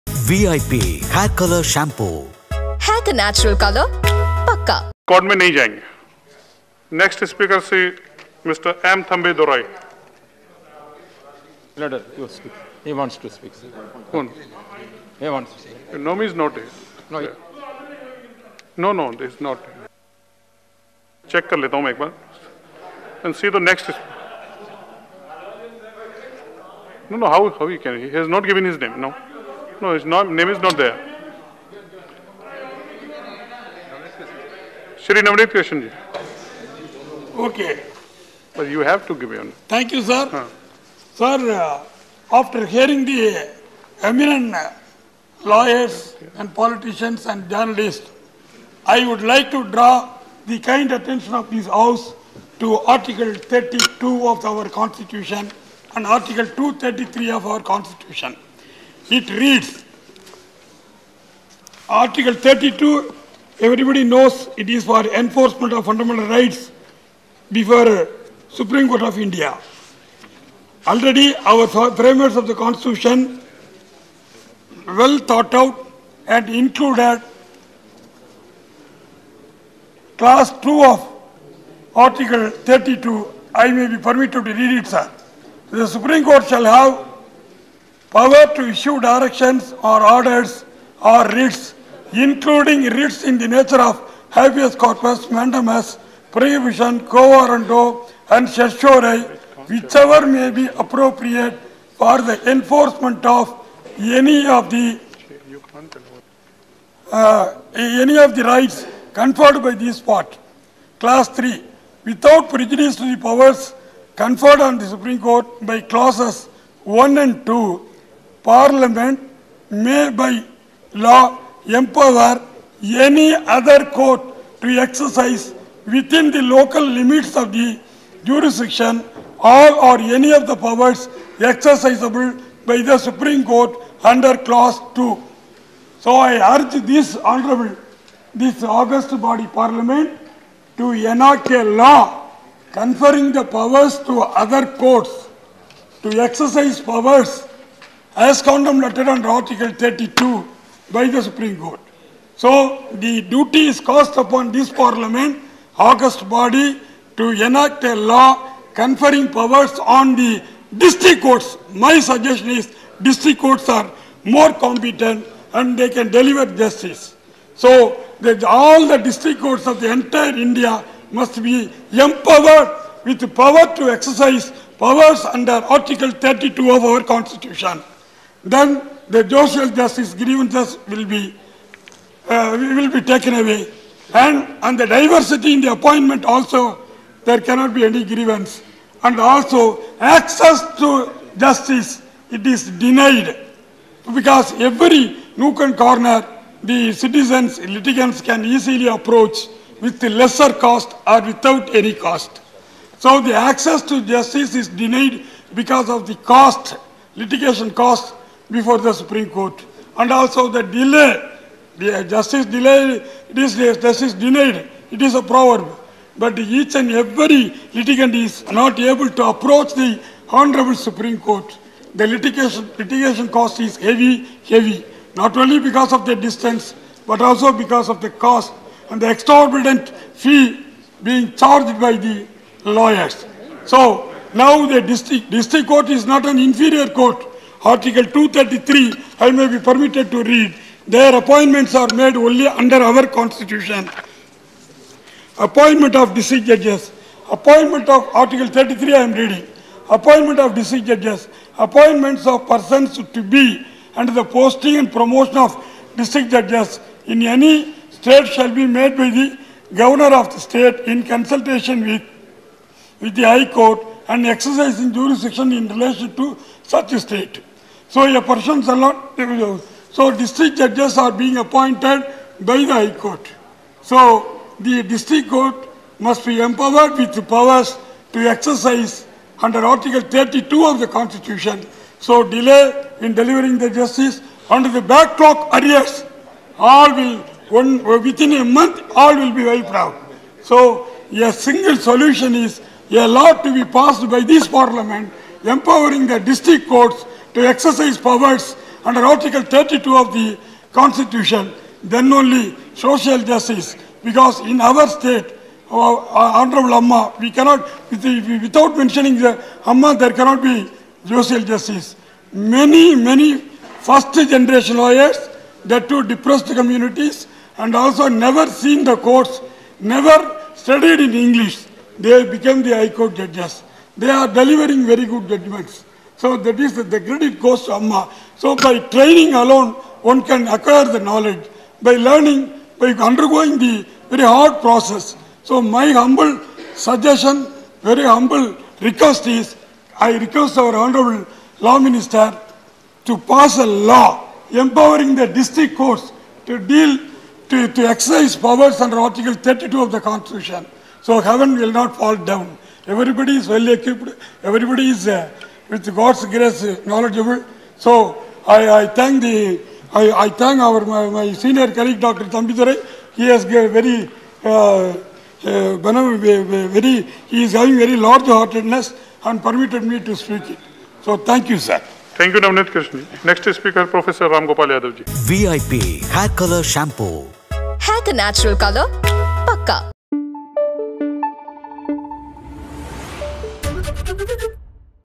Navaneethakrishnan Speech | பெயரே கொடுக்கல!எப்படி பேச முடியும்?சபாநாயகரை கடுப்பாக்கிய நவநீதகிருஷ்ணன்
Navaneethakrishnan Speech | பெயரே கொடுக்கல!எப்படி பேச முடியும்?சபாநாயகரை கடுப்பாக்கிய நவநீதகிருஷ்ணன் | AIADMK | Parliament Winter Session | Tamil News

Credit : Sansad TV